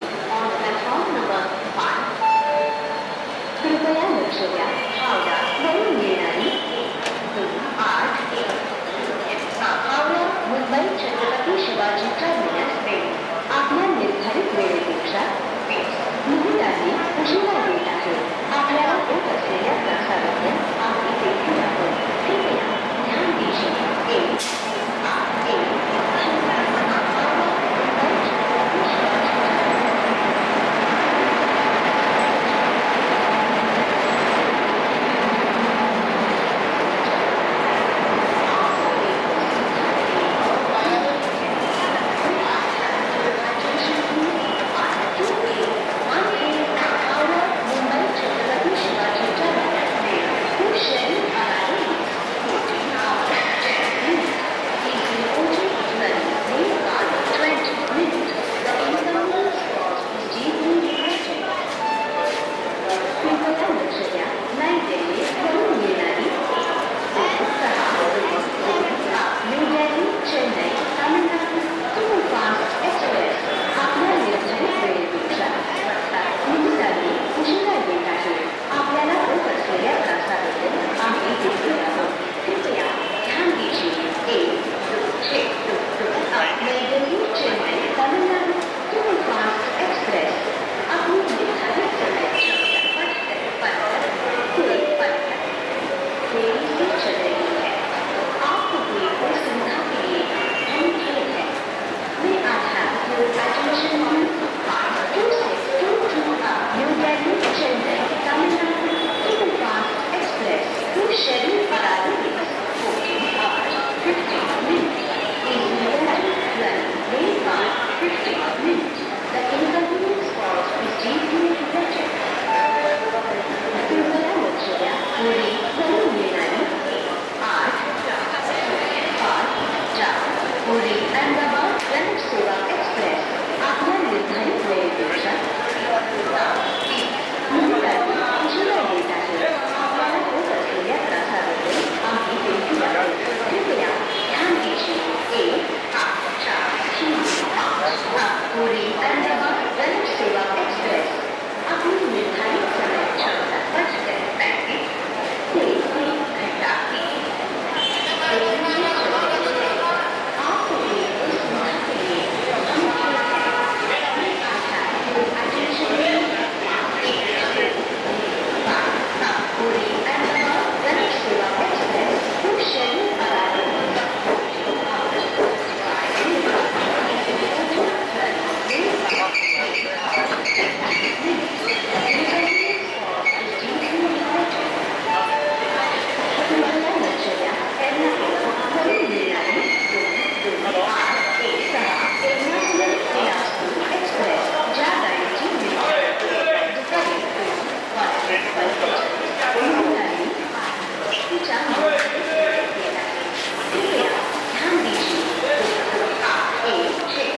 Chennai New Delhi Tamil Nadu Express led by Imposing 5350Hp ED WAP-4 arrives in style at Nagpur Jn.
Mighty ED WAP-4# 22219 arrives in stlye at Nagpur Jn with legendary superfast 12621 Chennai New Delhi Tamil Nadu Express. This train covers 2190kms (Chennai - New Delhi - Chennai) in up and down directions at average speed of above 65kph.

The other train in video is 12968 Jaipur Chennai Express with offlink LGD WAP-7.